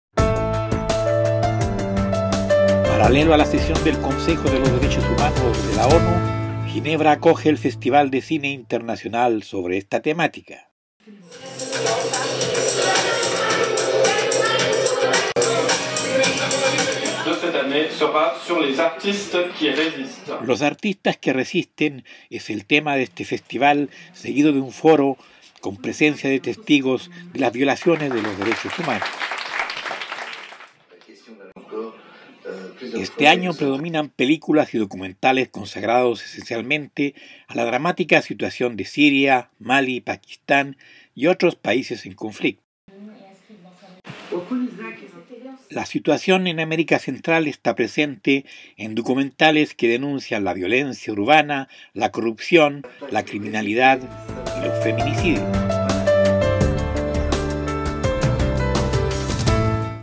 "Los artistas resisten" tema del Festival Internacional de Cine sobre Derechos Humanos
"Los artistas resisten", es el tema de la 11° edición del foro y festival de filmes sobre la problemática de los Derechos Humanos que se celebra en Ginebra.